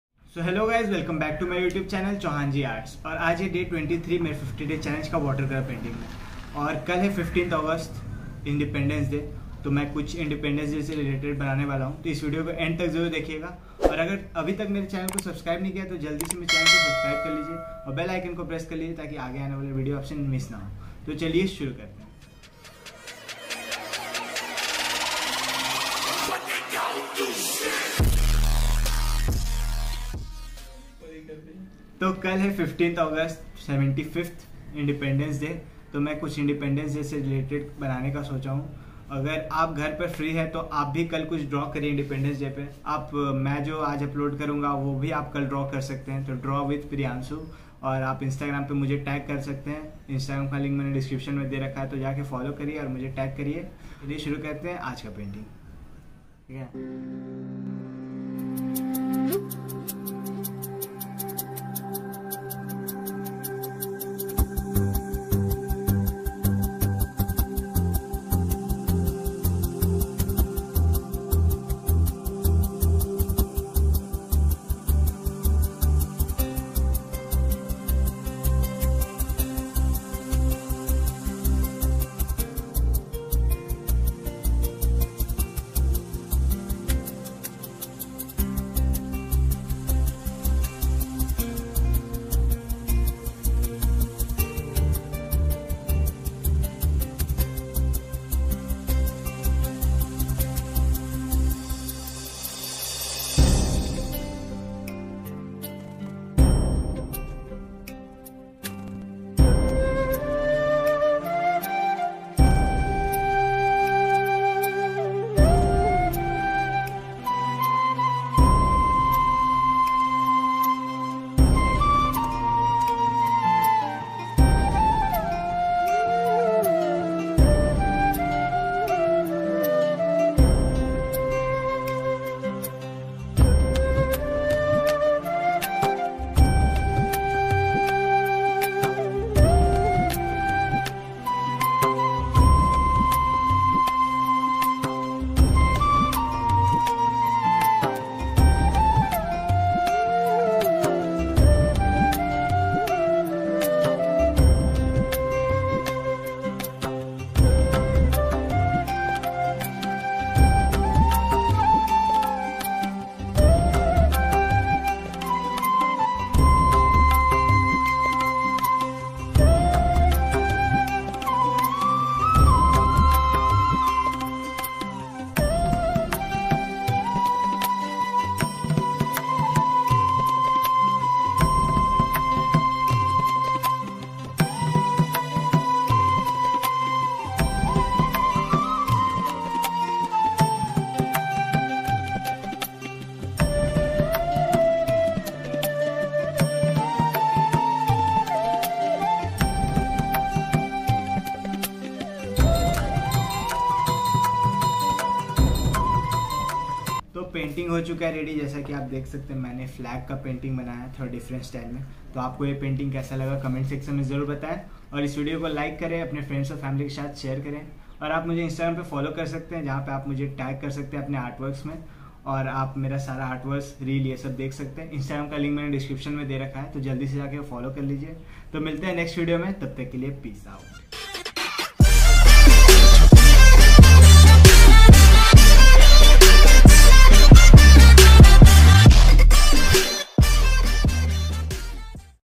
Independence day drawing with watercolor🇮🇳❤️ | Independence day painting | Day23 | 50 Days challenge
HAPPY INDEPENDENCE DAY ❤️🇮🇳happyindependenceday
 painting for beginners 🖌️🎨. Start painting today with me and join me on my 50Days challenge 💪

Technical pen
Mobile Holder
brush: